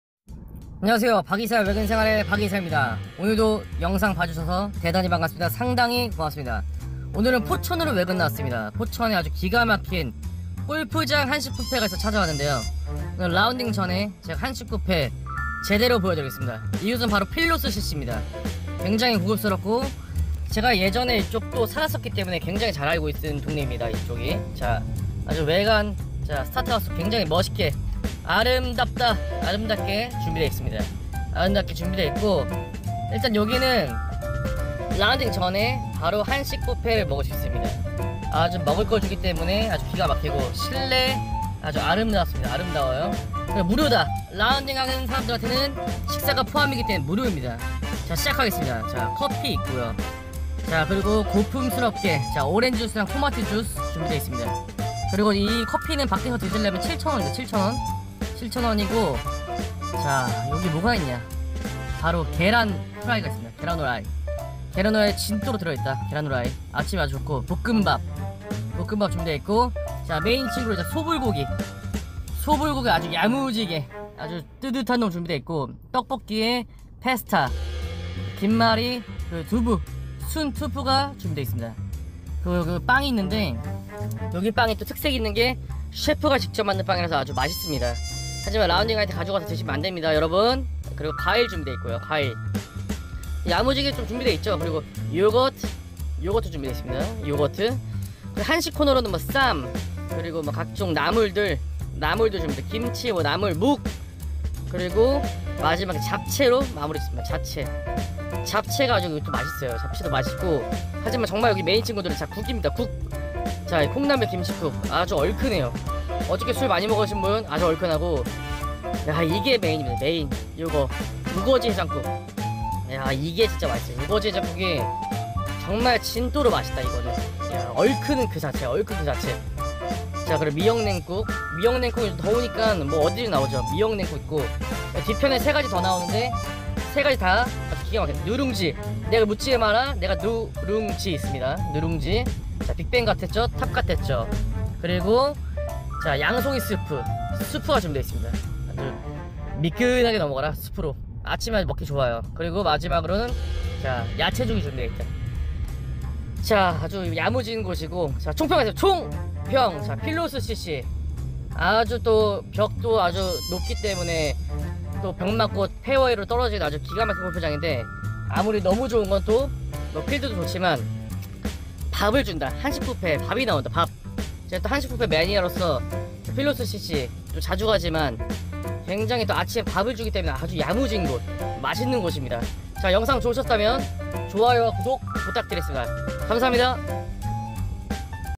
골프장 한식뷔페 끝판왕!!!
안녕하세요 박이사 입니다.
오늘은 포천에 위치한 아주 풍경이 아름다운
골프장 필로스CC 다녀왔습니다.
이 곳에 아주 기가 막힌 한식뷔페가 있어서
소개 시켜드립니다!!

[가게정보]
필로스GC
경기도 포천시 일동면 운악청계로 1507